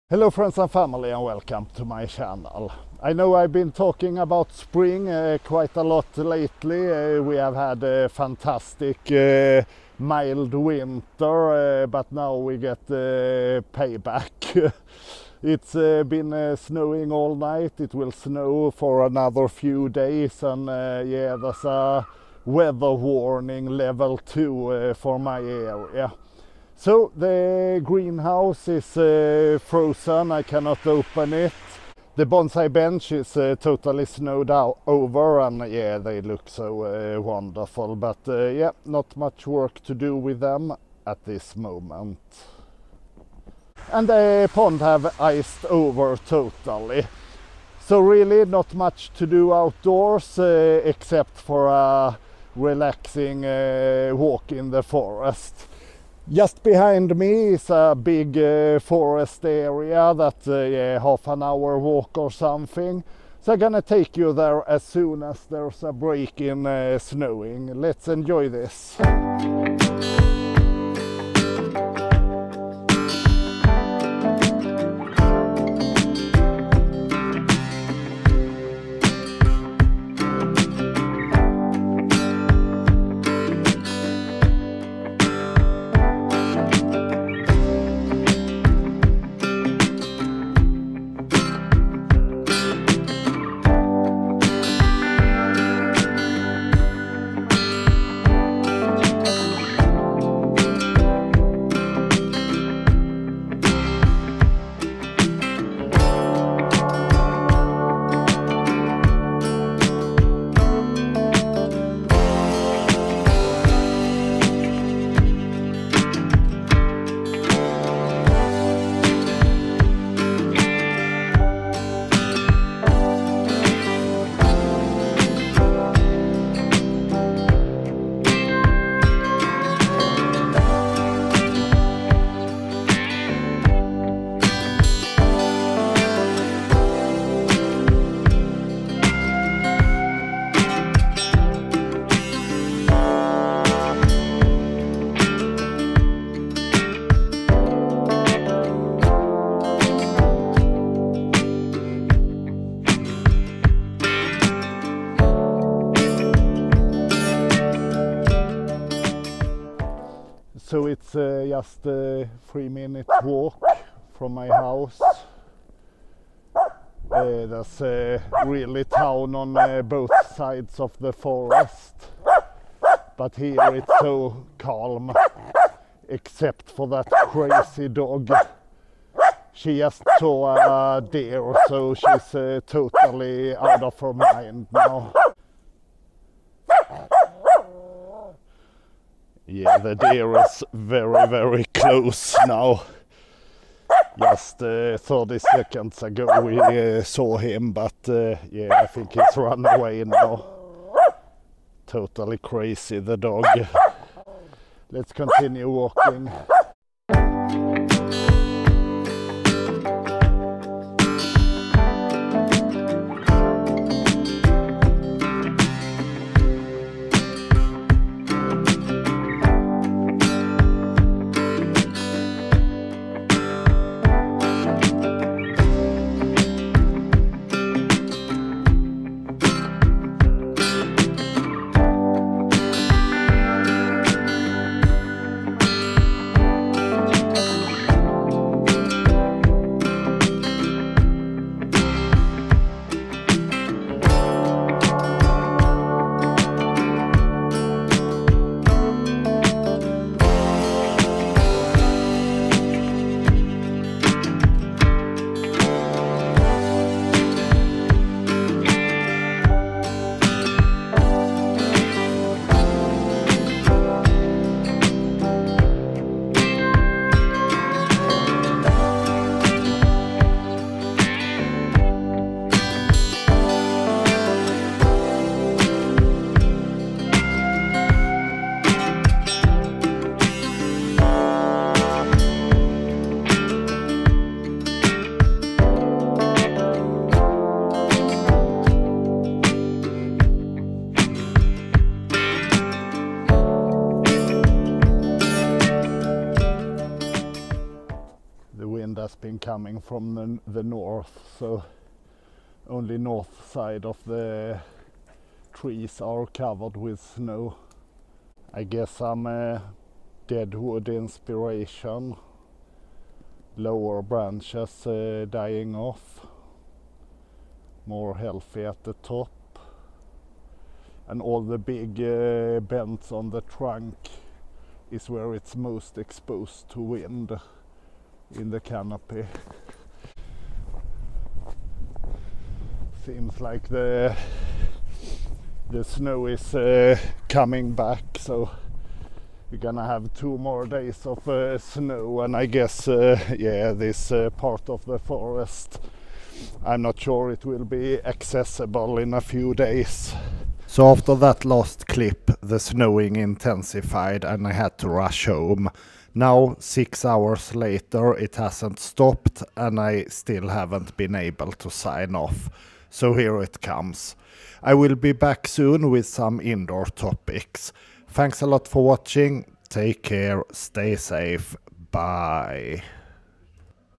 Nature adventure and bonsai inspiration when spring is on hold
This is my local forest covered in snow. The nature types here ranges from nutrition rich deciduous forest, bogs, alpine habitat to nutrition pour coniferous forest. All within 15 minutes work from my back yard.